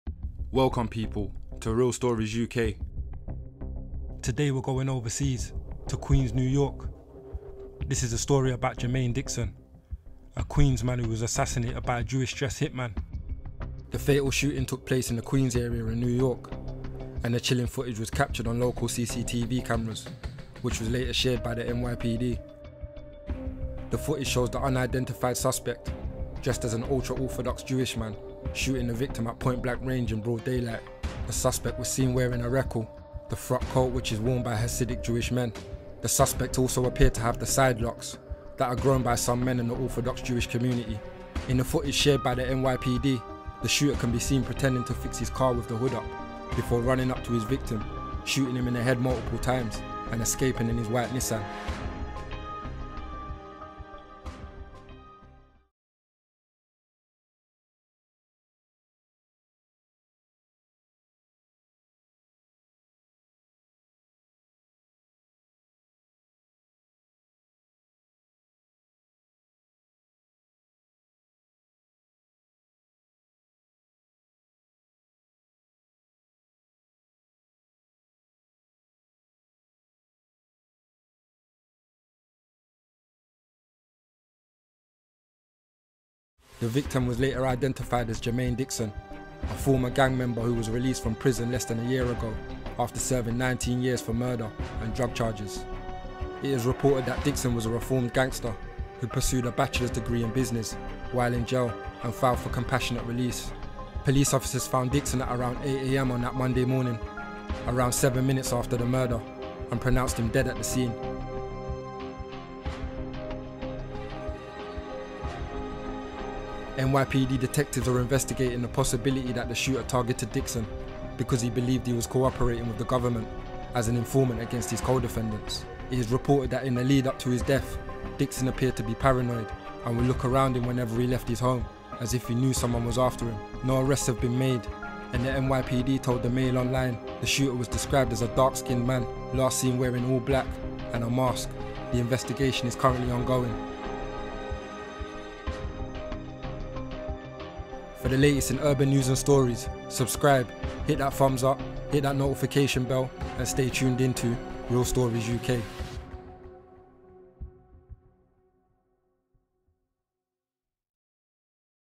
Story Time | Hasidic Dressed Hitman Shoots Man Dead In Queens New York | Real Stories UK
Queenz man shot dead by Jewish dressed hitman for allegedly working with the government against his co-defendants.